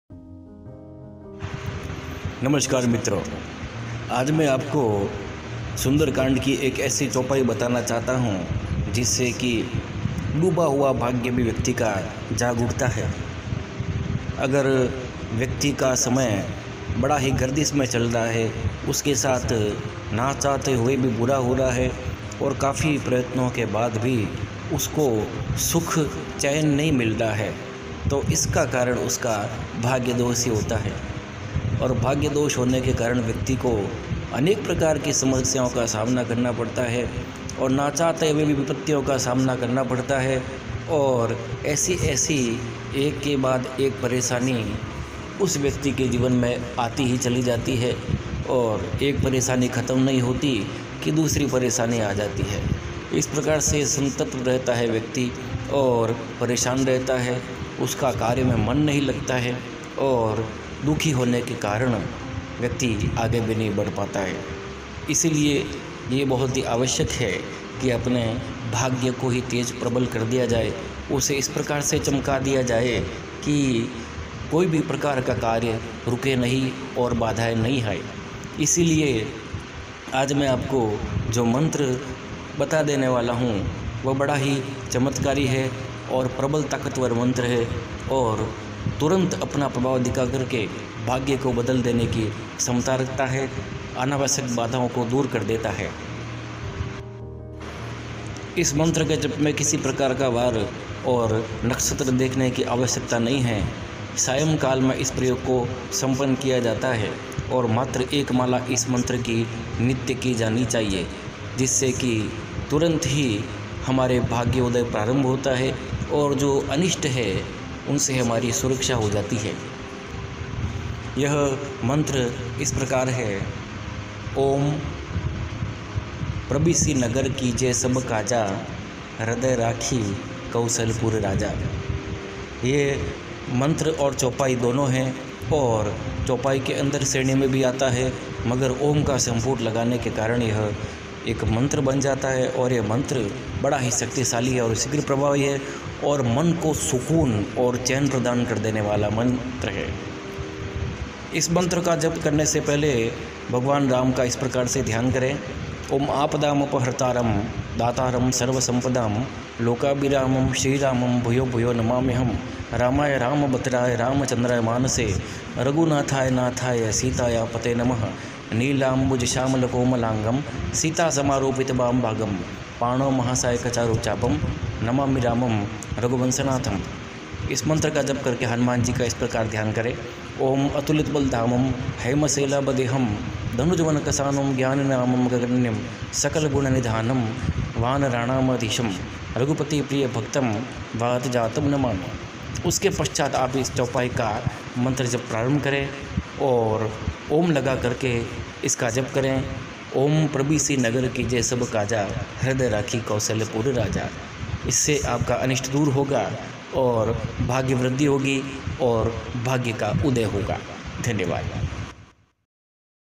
तुरंत किस्मत जगाने और भाग्य बदलने की चमत्कारी चौपाई
तुरंत सोई हुई तकदीर को जगाने और भाग्य को बदलने का प्राचीन नुस्खा है ये मंत्र ||